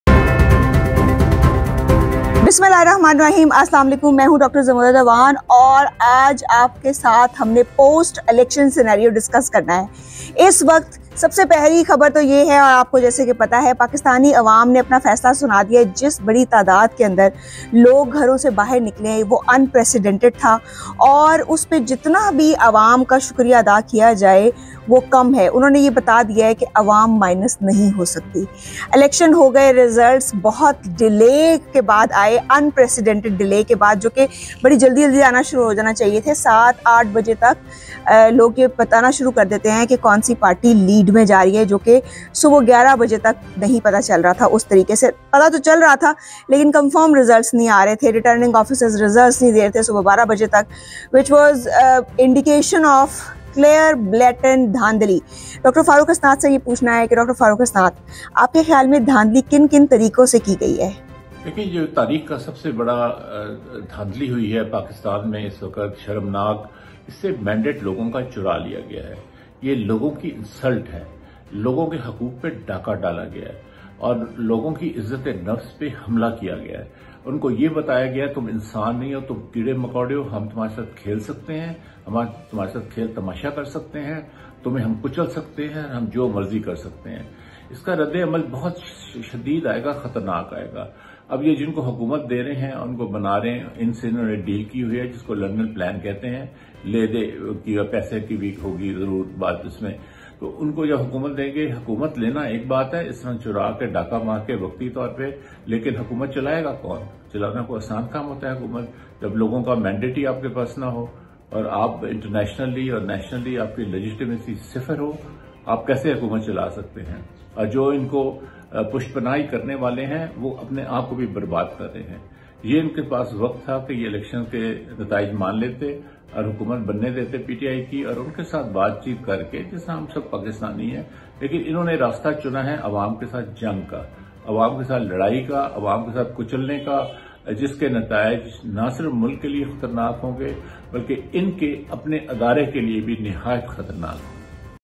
2024 Elections – Shameless, Rigging by Evil Forces - Dr Farooq Hasnat & Dr Zamurrad - Feb 09,2024
A Day after 2024 Elections – Shameless, Massive Rigging by Evil Forces
انتخابات کے ایک دن بعد - شیطانی قوتوں کی بے شرمی، بڑے پیمانے پر دھاندلی 2024

DR FAROOQ HASNAT & ڈاکٹر فاروق حسنات
DR ZAMURRAD AWANڈاکٹر زمرد أعوان
FEB 09,2024